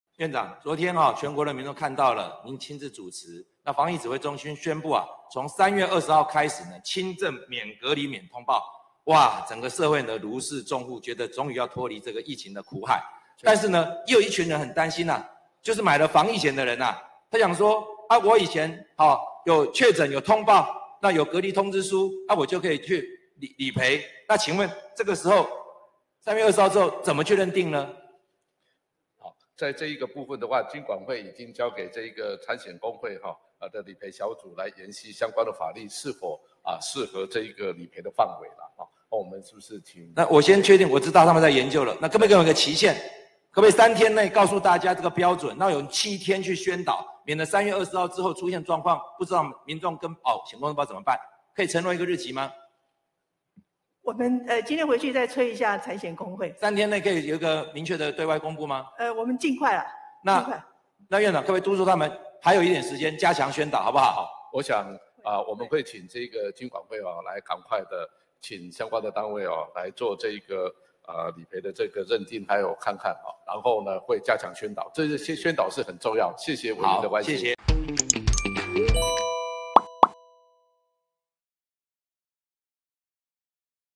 輕症免隔離「防疫險」賠不賠？陳建仁：請金管會做理賠認定【CNEWS】
CNEWS匯流新聞網記者 陳儀潔 韓羽婕 李文成 王聿瀠 劉宇軒 陳巧紜 尹佳嵐／台北報導

📍完整直播
【2023/03/10 立法院會議 | 國會頻道-立法院議事轉播】